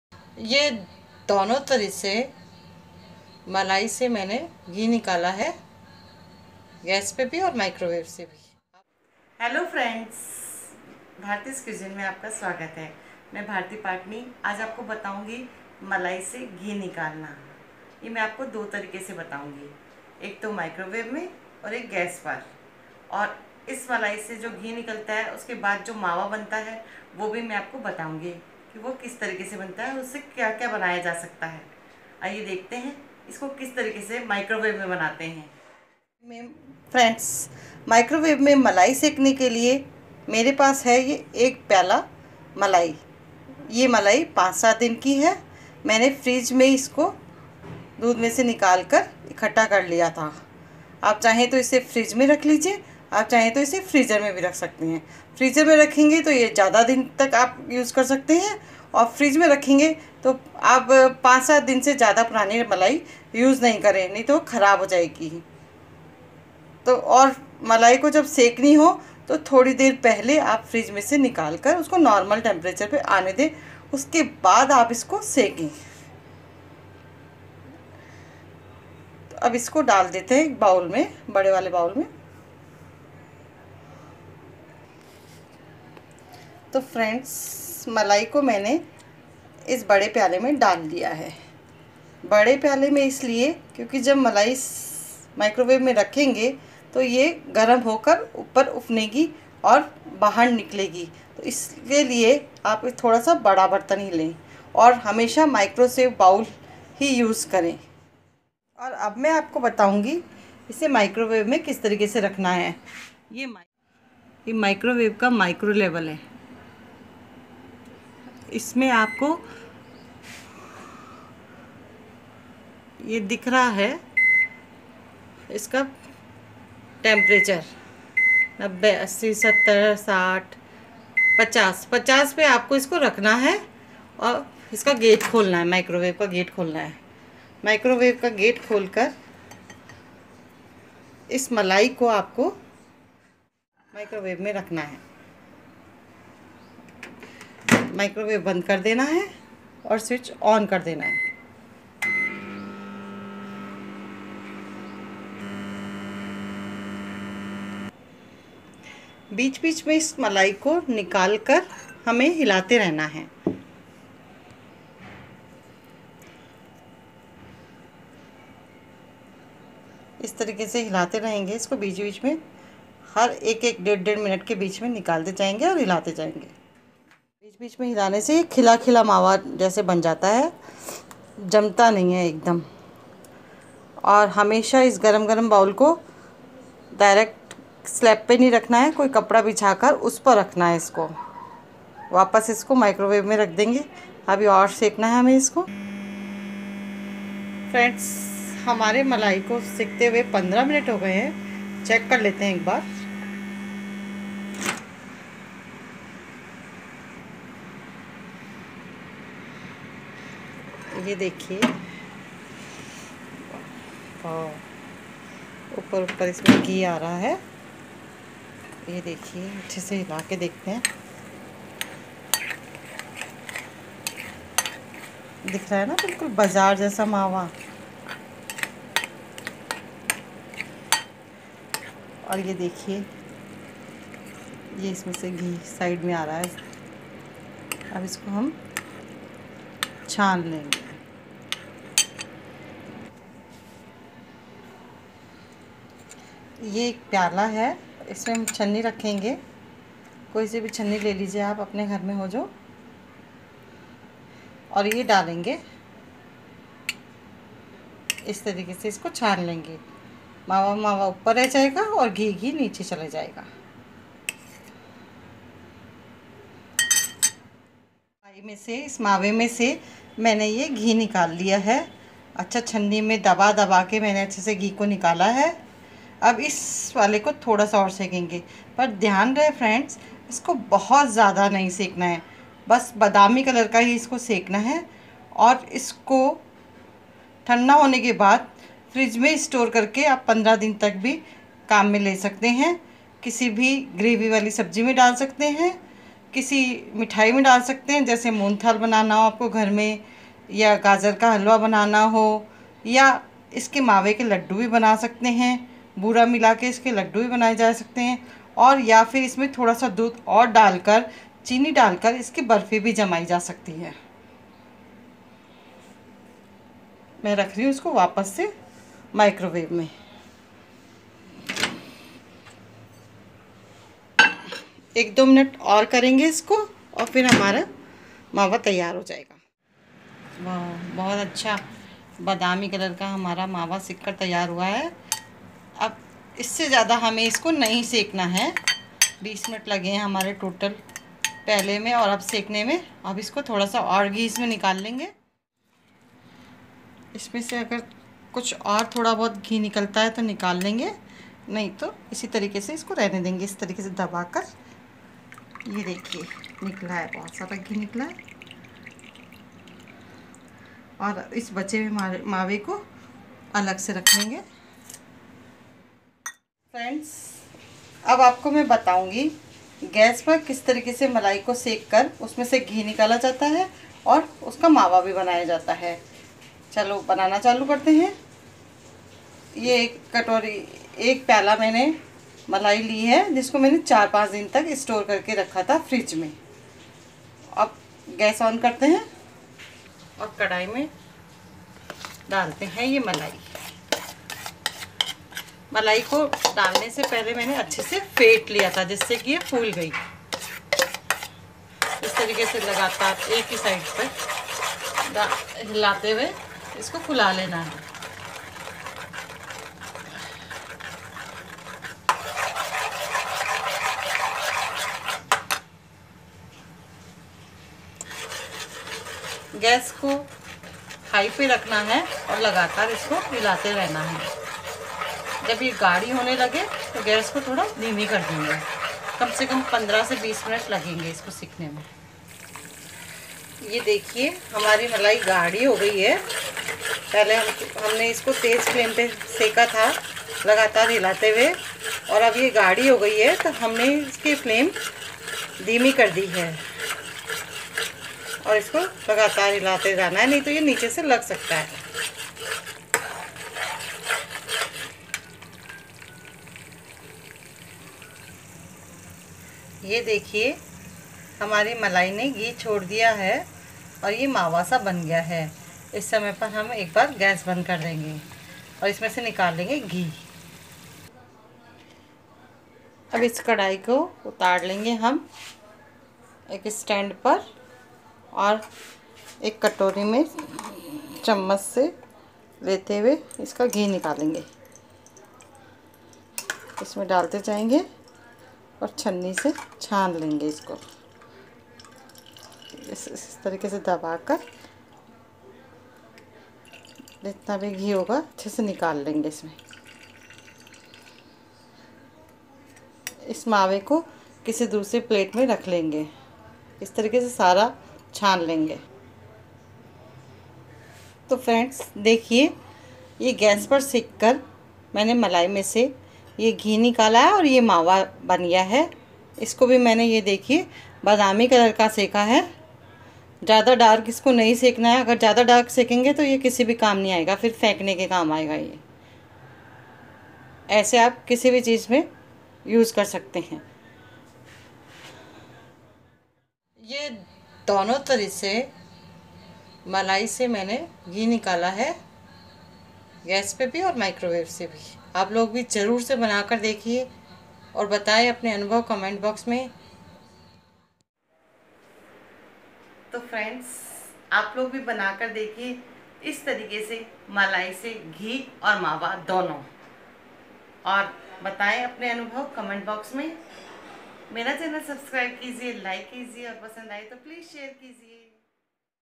मलाई से घी और मावा l Malai se Ghee & Mawa l Microwave mai bhi | Bharti Patni
अब नहीं फेंकना मलाई से निकला Mawa ,थोड़ी सी सावधानी से अगर सकेंगे इसे तो मिलेगा मावा भी,
आम के आम गुठलियों के दाम यह है ,हमारी इस रेसिपी की विशेषता, हम निकालेंगे मलाई से घी भी और उससे मावा भी और फिर बनाएंगे बेहतरीन मिठाइयां l
15 - 20  मिनट में बनाएंगे यह रेसिपी  अपने घर में अपने हाथों से l